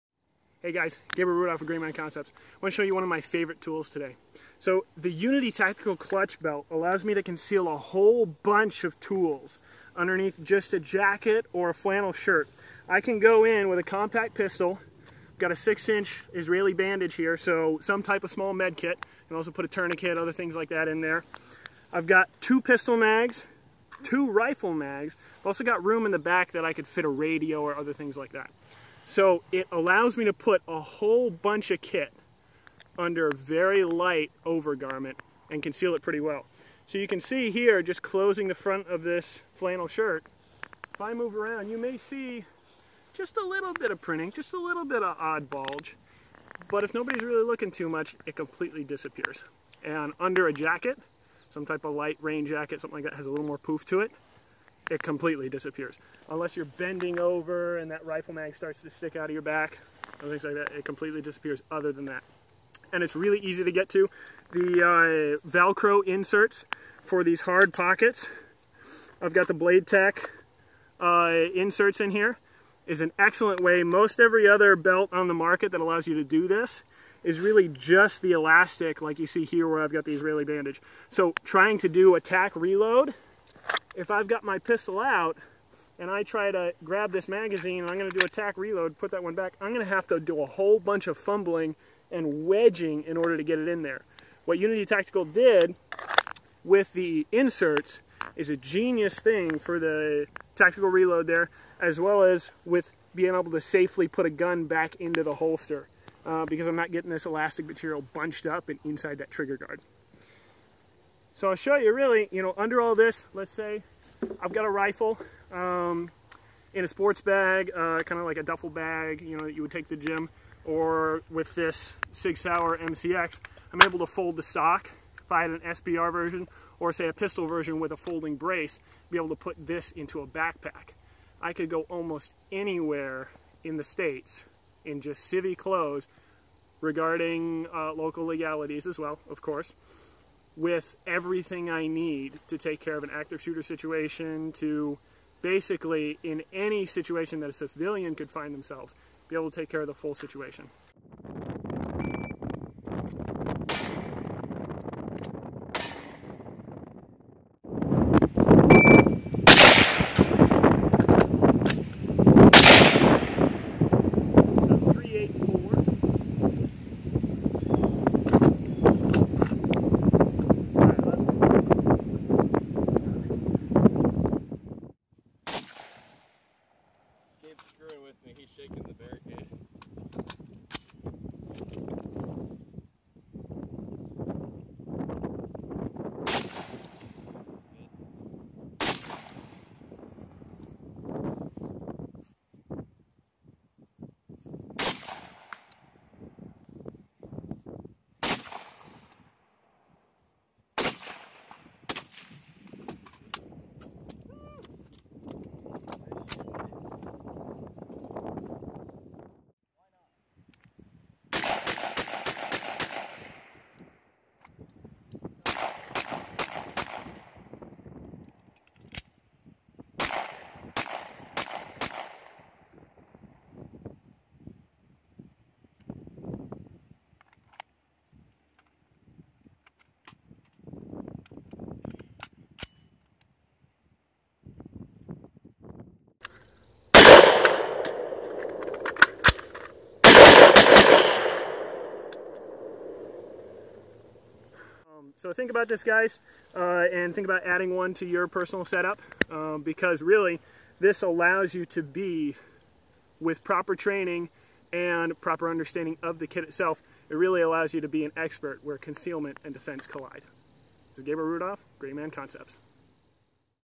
Unity Tactical Clutch Belt review - The Greyman's favorite tool
Quick review of the Unity Tactical Clutch Belt. More to come soon.